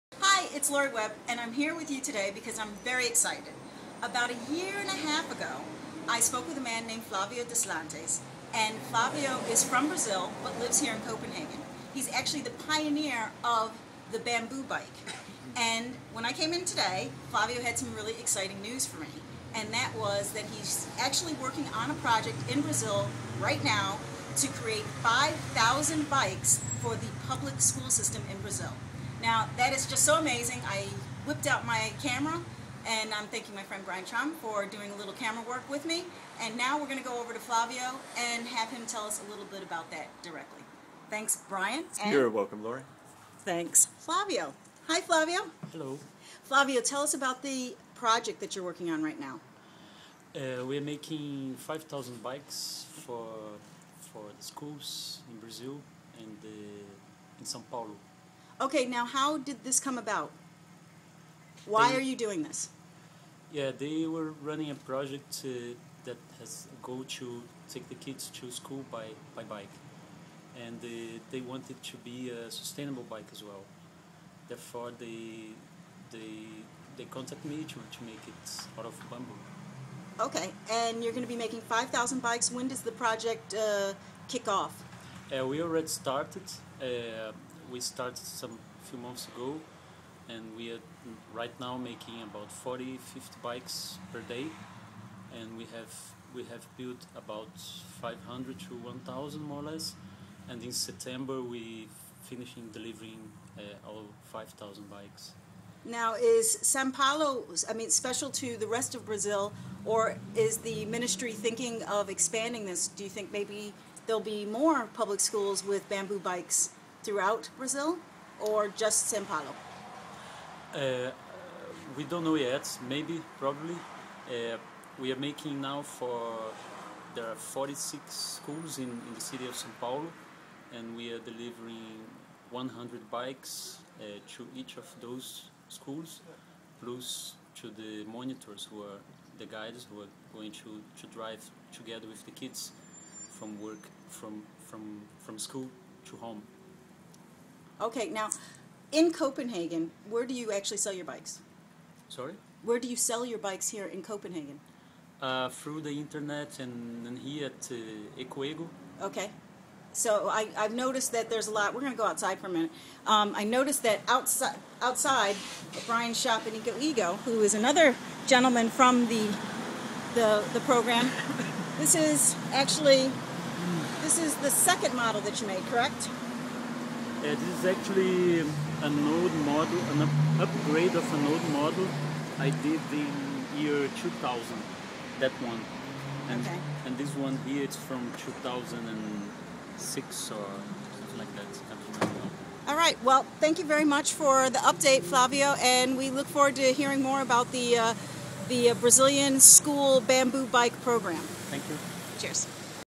Bamboo Bikes for Sao Paulo Public Schools
Flavio Deslandes is a Pioneer in Bamboo Bike Design, one of the featured entrepreneurs in the book, Entrepreneurial Profiles:Around the World & Down the Street and is currently working on a project for the Municipality of Sao Paulo to create 5,000 Bamboo Bikes for their Public School System.